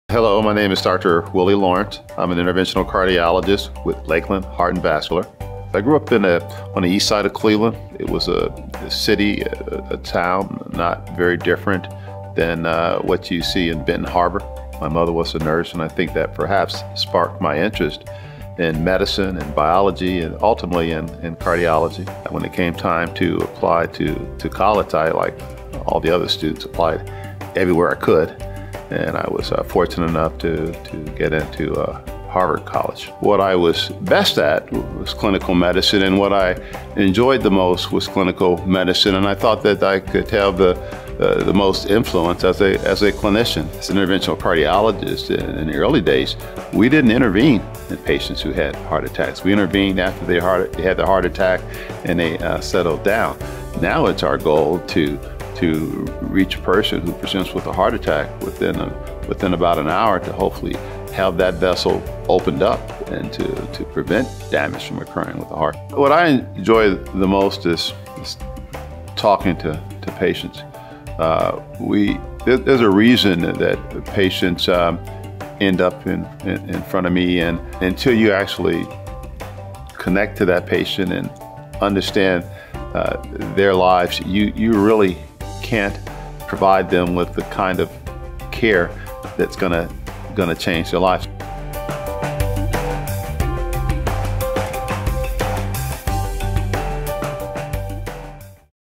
Physician Video Profile: William Lawrence, MD (Cardiology)
William Lawrence, MD is an interventional cardiologist with Spectrum Health Lakeland that specializes in adult cardiovascular disorders.